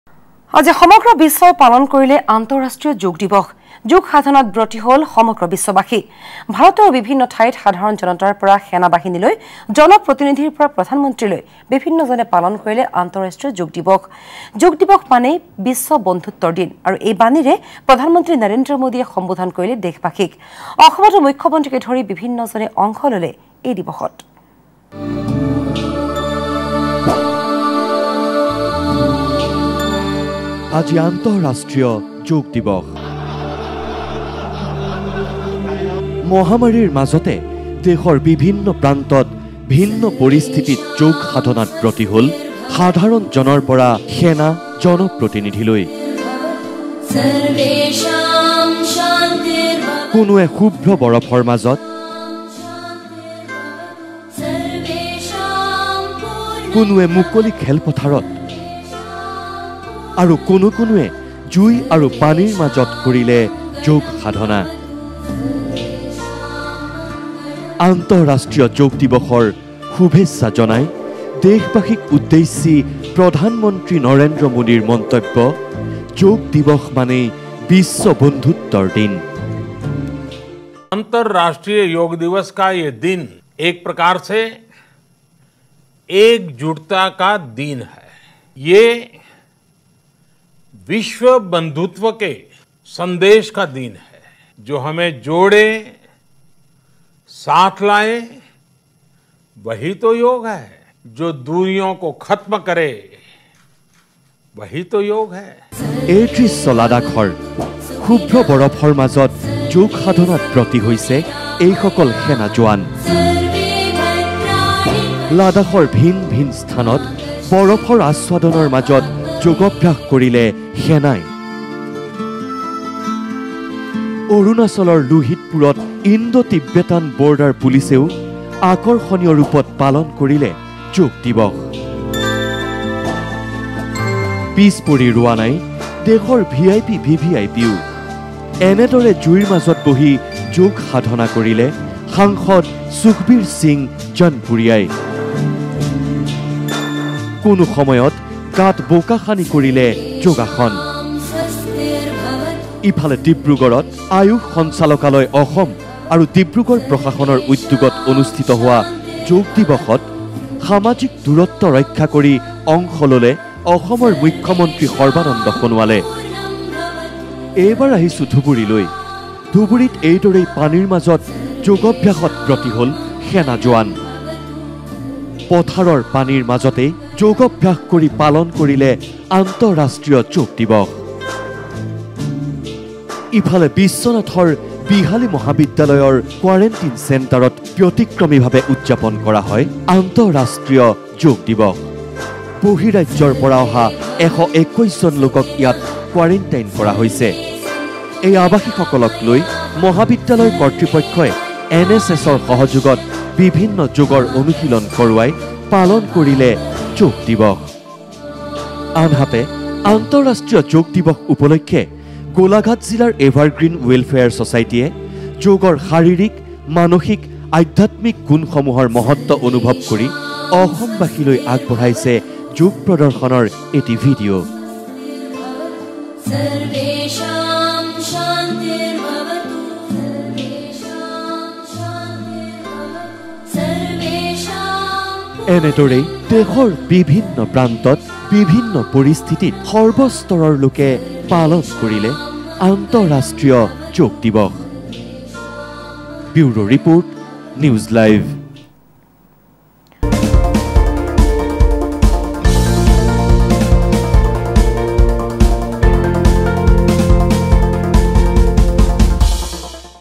সমগ্ৰ বিশ্বই আজি পালন কৰিলে আন্তঃৰাষ্ট্ৰীয় যোগ দিৱস
আজি সমগ্ৰ বিশ্বই পালন কৰিলে আন্তঃৰাষ্ট্ৰীয় যোগ দিৱস। যোগ সাধনাত ব্ৰতী হ’ল সমগ্ৰ বিশ্ববাসী। ভাৰতৰো বিভিন্ন ঠাইত সাধাৰণ জনতাৰ পৰা সেনা বাহিনীলৈ, জনপ্ৰতিনিধিৰ পৰা প্ৰধানমন্ত্ৰীলৈ বিভিন্নজনে পালন কৰিলে আন্তঃৰাষ্ট্ৰীয় যোগ দিৱস। যোগ দিৱস মানেই বিশ্ব বন্ধুত্বৰ দিন। এই বাণীৰে প্ৰধানমন্ত্ৰী নৰেন্দ্ৰ মোদীয়ে সম্বোধন কৰিলে দেশবাসীক। অসমতো মুখ্যমন্ত্ৰীকে ধৰি বিভিন্নজনে অংশ ল’লে এই দিৱসত।